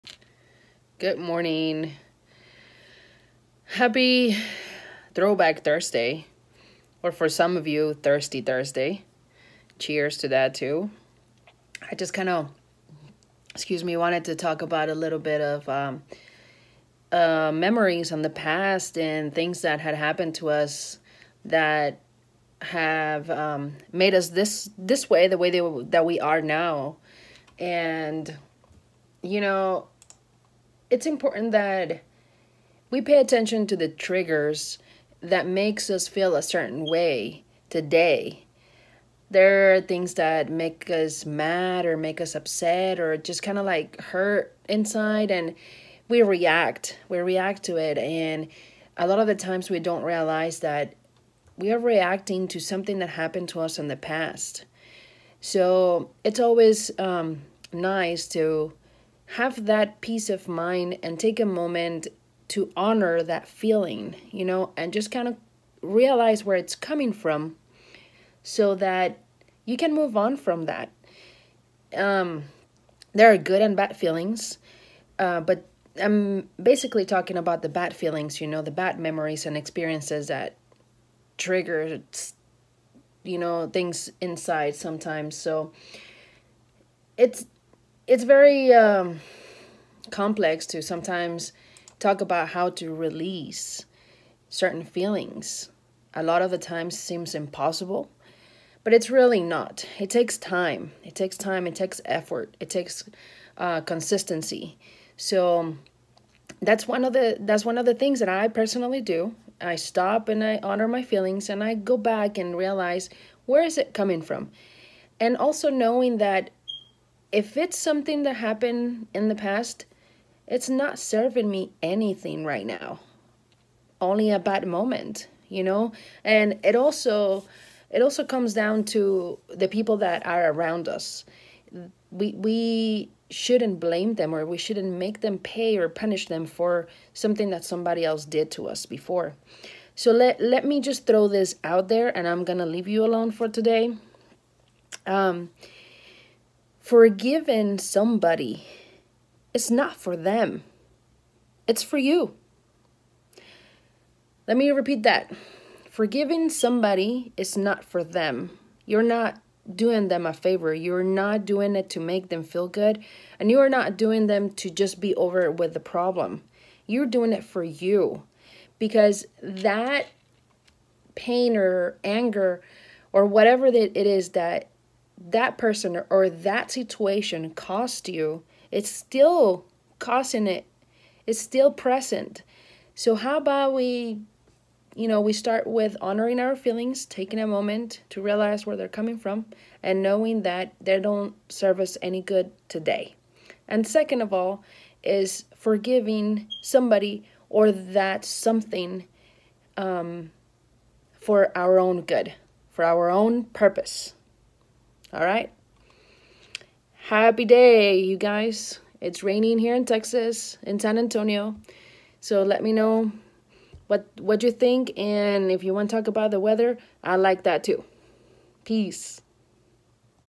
TBT to your feelings and emotions!
Small steps towards releasing emotions and feelings that no longer serve us. Be thankful for the lesson but don’t drag along memories that cause you pain or anger. Work constantly on you first. Stay grounded and aware! Peace.